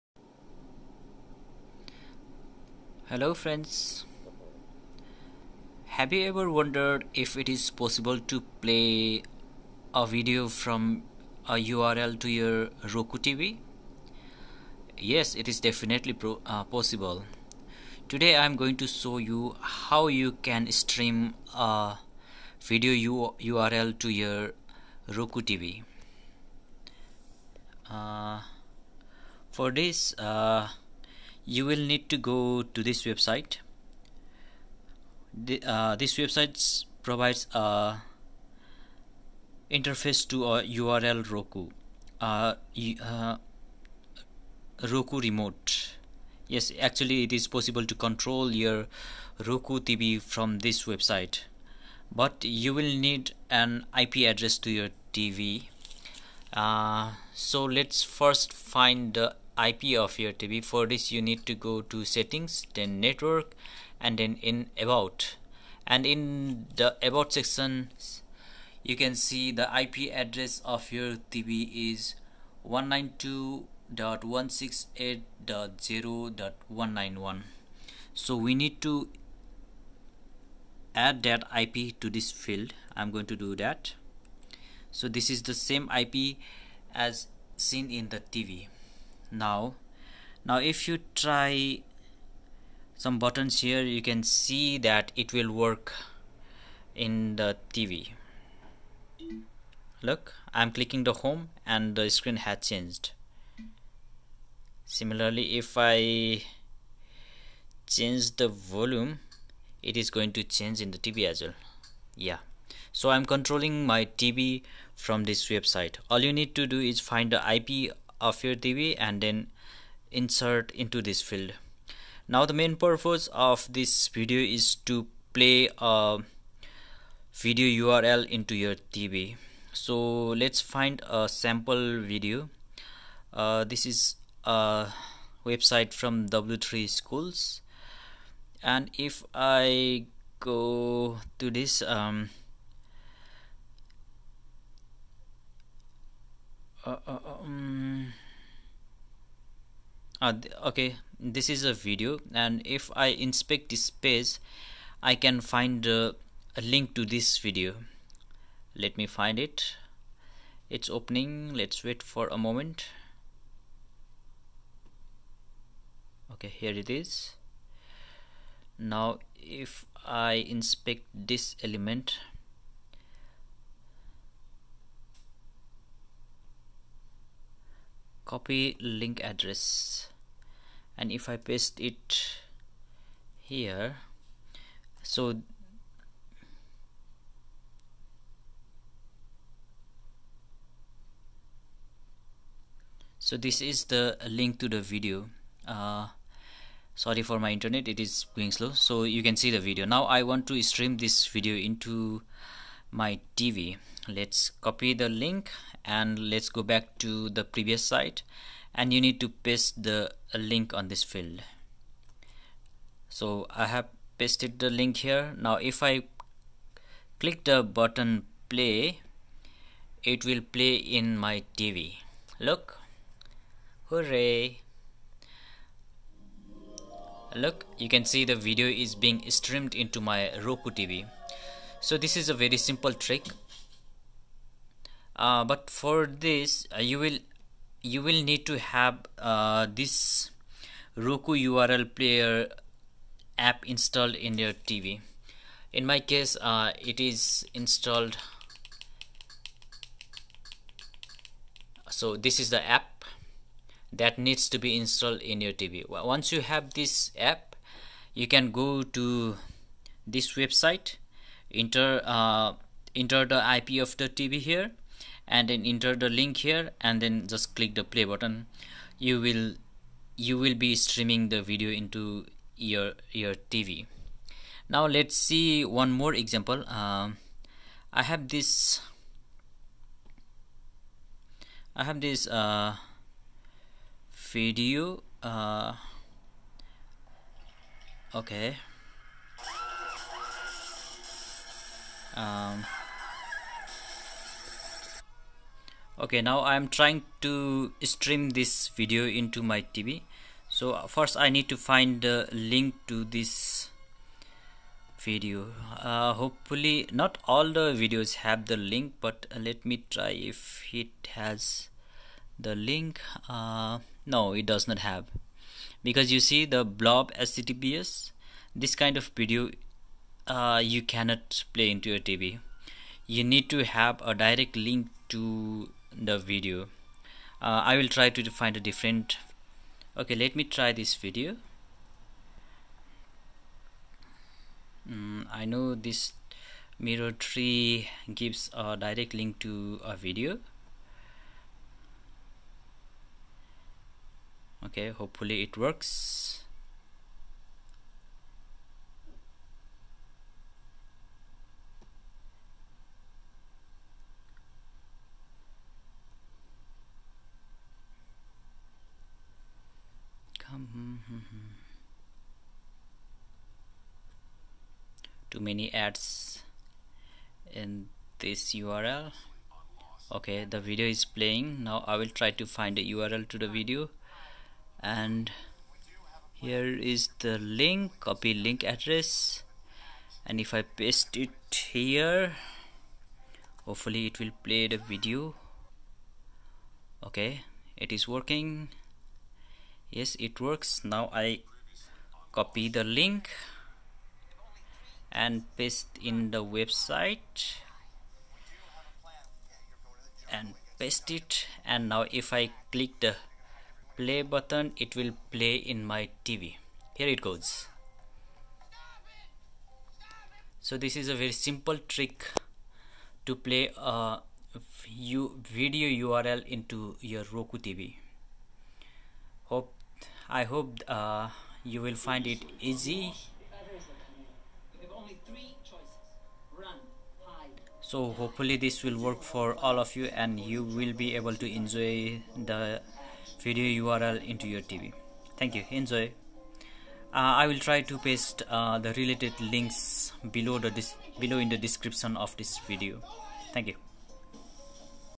Stream Video Link in your ROKU TV | How to play video URL in your Roku TV?
In this video I am trying to demonstrate how you can play a video url in your roku tv.

For this to work you will need to install URL Player app in your roku tv.